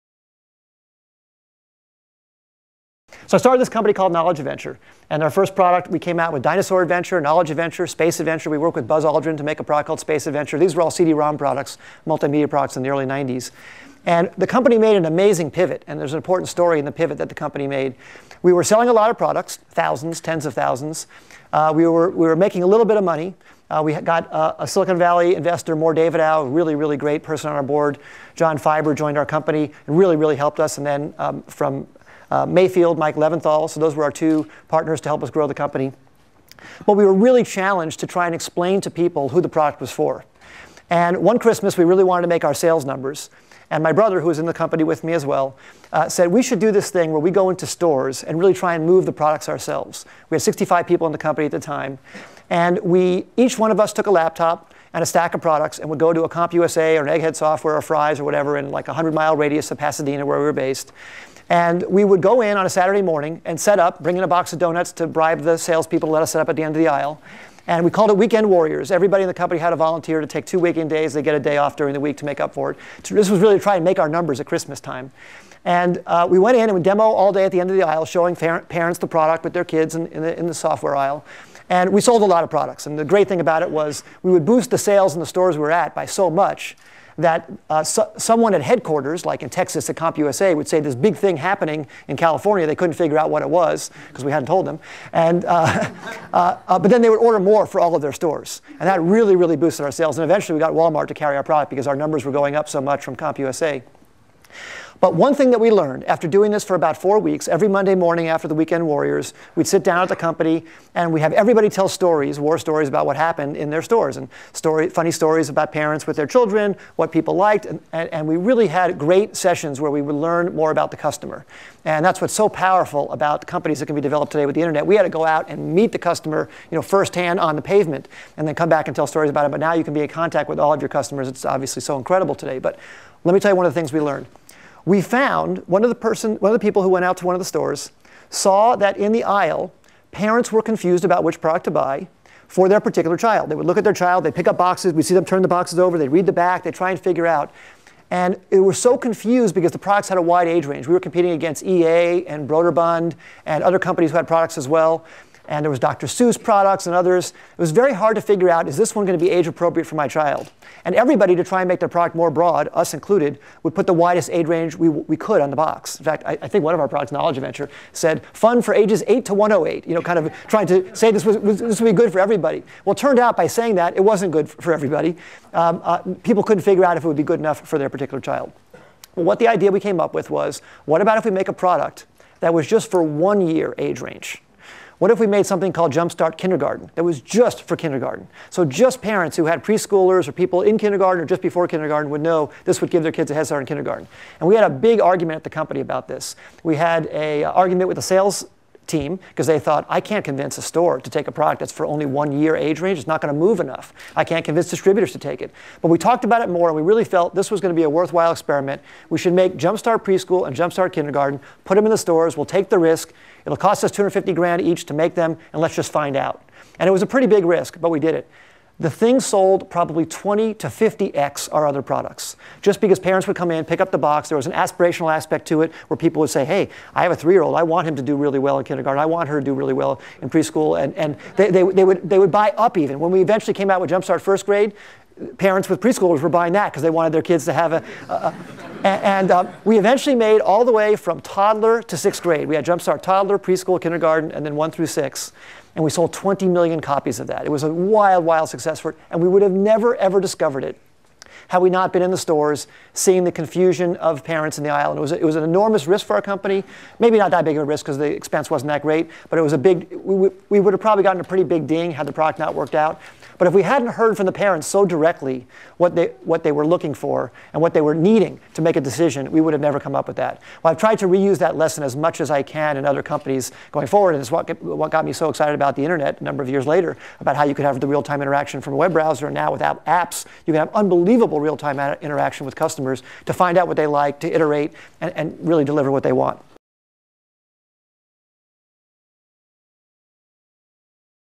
Bill Gross: Weekend Warriors Discover the Pivot
Before Bill Gross founded Idealab, he started Knowledge Adventure, an early educational software company. In this clip, Gross shares how Knowledge Adventure discovered its major pivot through direct interaction with customers. In an effort initially meant to reach sales targets, employees served as a "weekend warriors" demonstrating products in retail outlets. This effort revealed the need to target their software products to specific ages, a key insight to the company's future success.